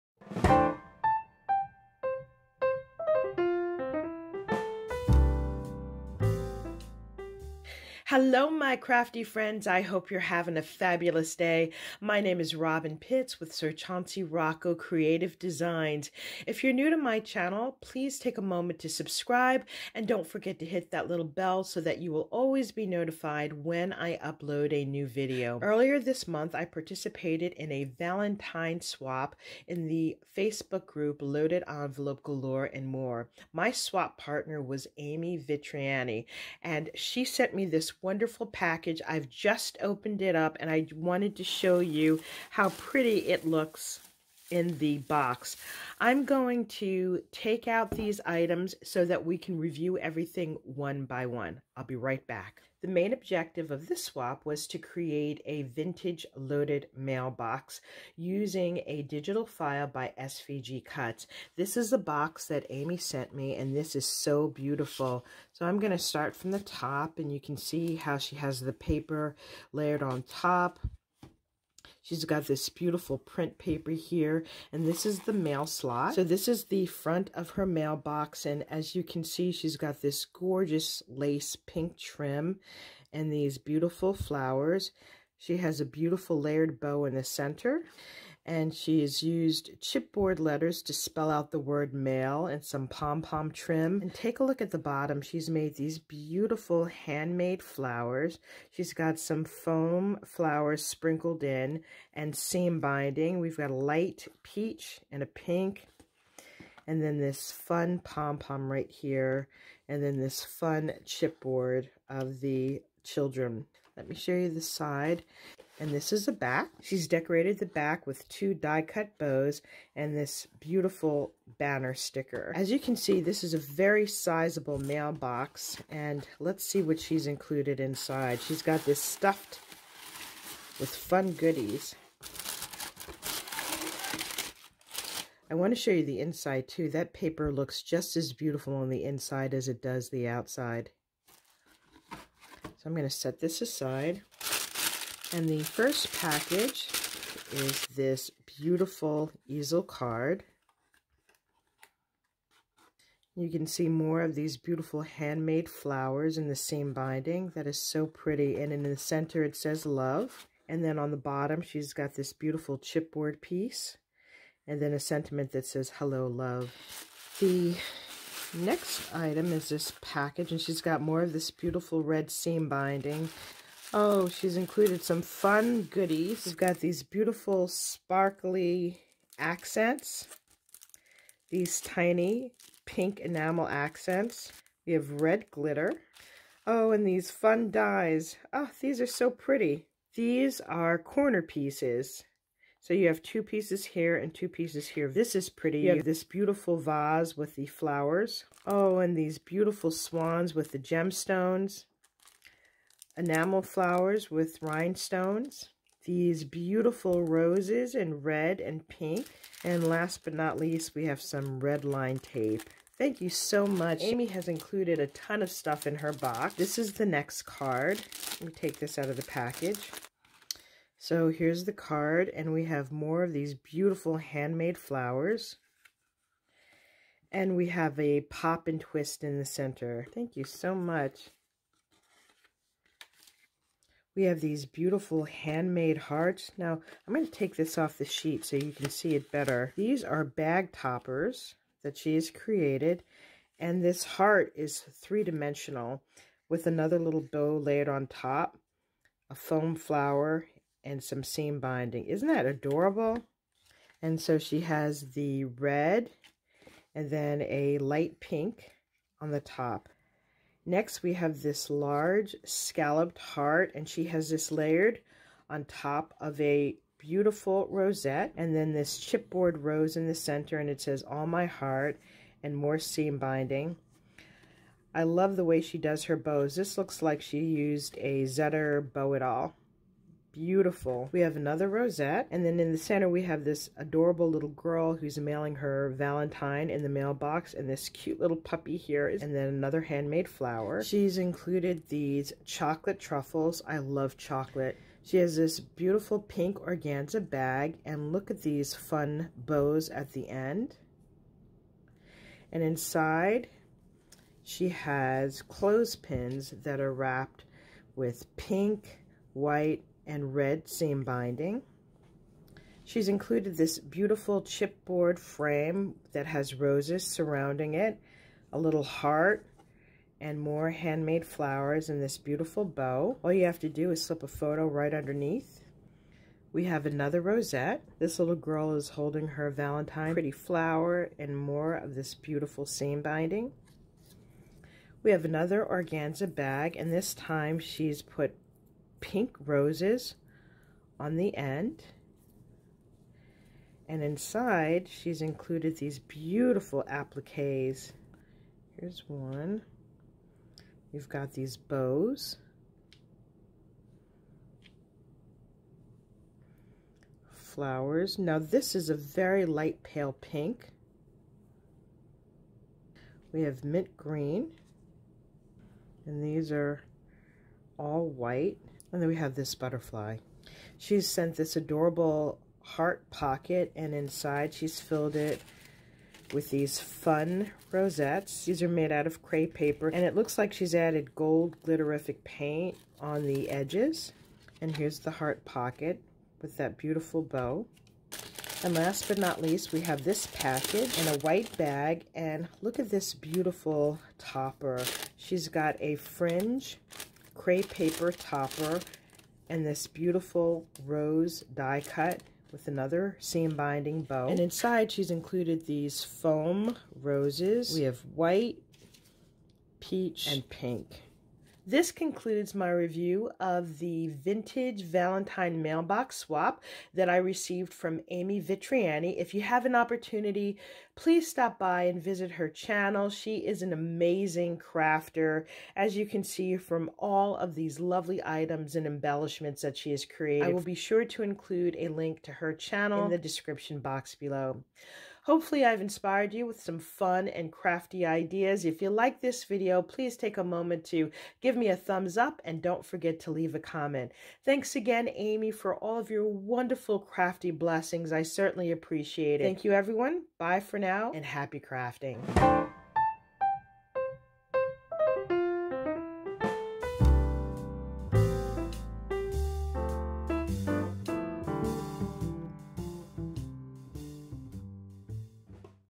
Valentine Mailbox Swap from Amy Vitriani | Loaded Envelope Galore & More
Hello Crafty Friends, I received this amazing Valentine Mailbox Swap from Amy Vitriani!!! This swap was hosted by Loaded Envelope Galore & More on Facebook. Amy’s package is filled with tons on Valentine crafty goodness. Please be sure to check out Amy’s YouTube channel

Robyn M. Pitts,
Sir Chauncey Rocco Creative Designs